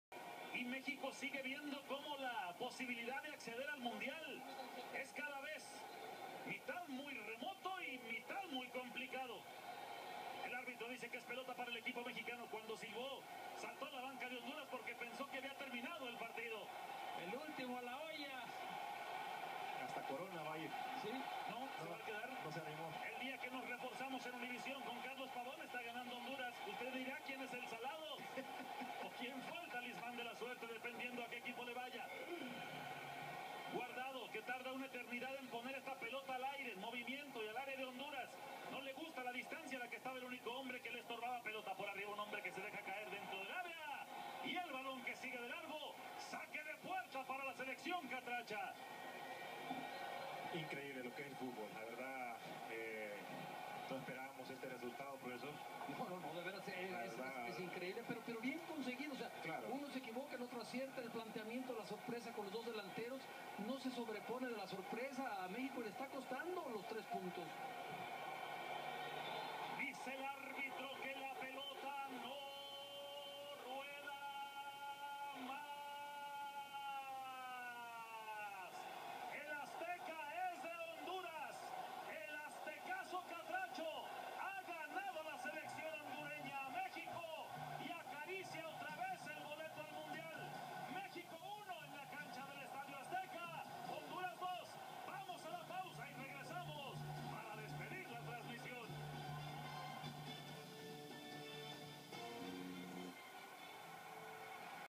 Honduras 2 Vs Mexico 1
No Es El Aztecazo Sencillamente Honduras Le Gano A Mexico Viernes 6 De Sept 2013